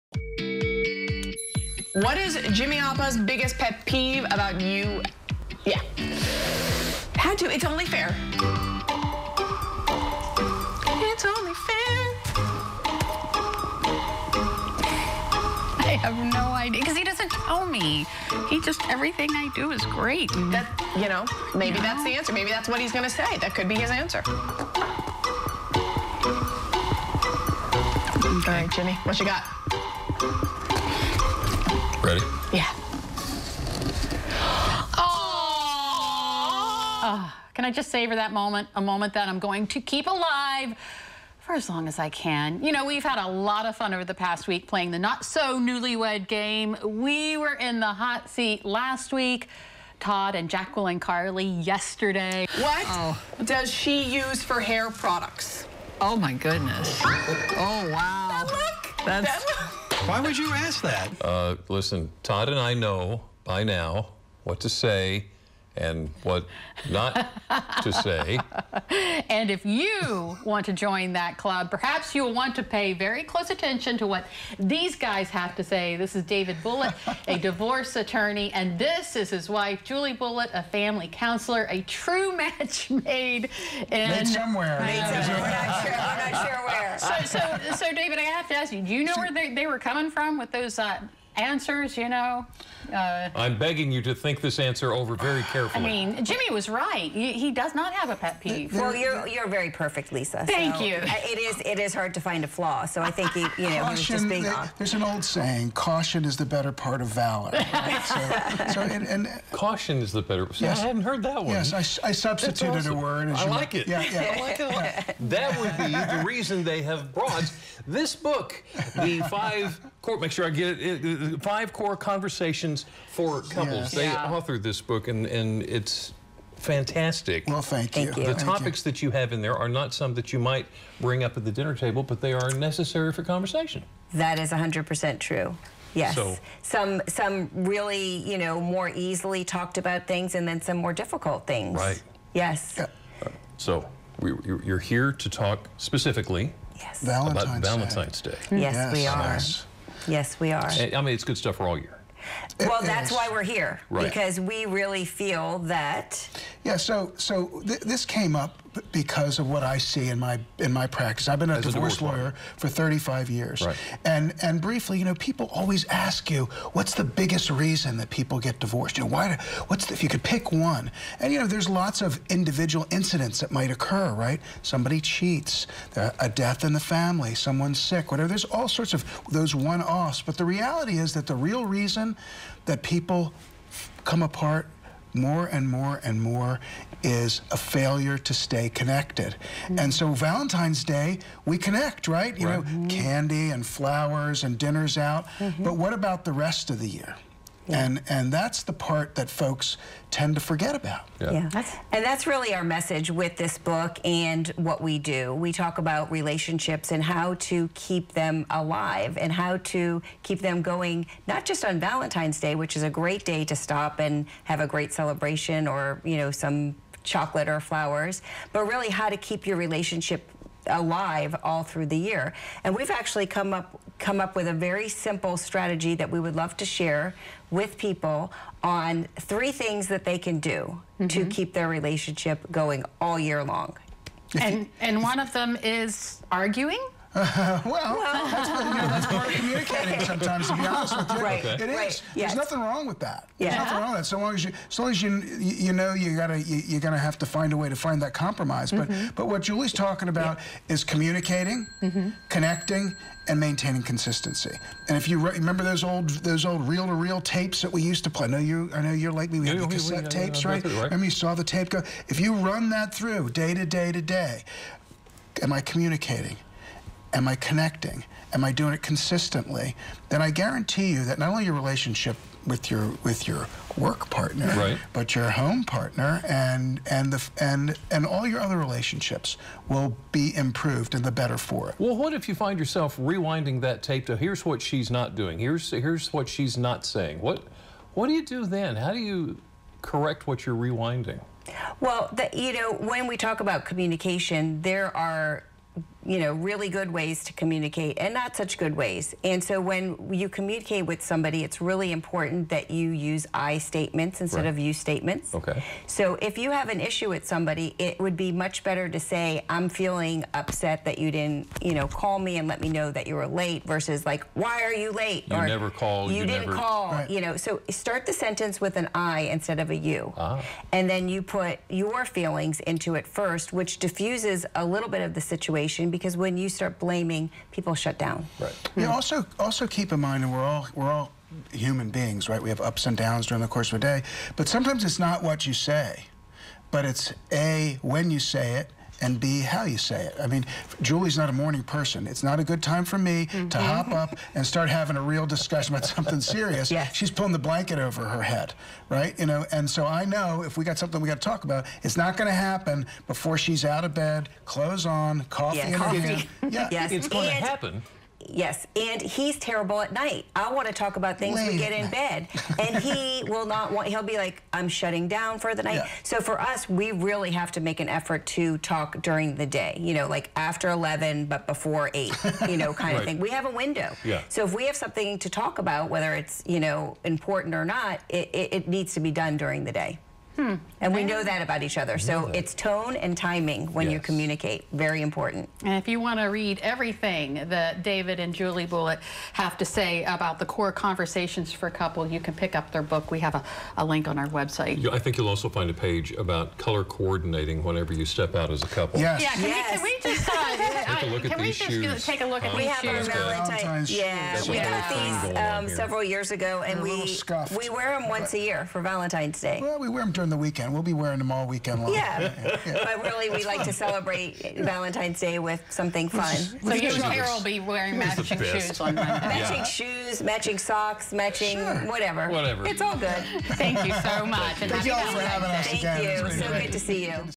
We Sit Down With The Bulitts To Hear Their Advice on How To Keep The Love Alive All Year Long
We're sitting down with the authors of "The Five Core Conversations for Couples," David and Julie Bulitt. They talk about how to keep the love alive all year long.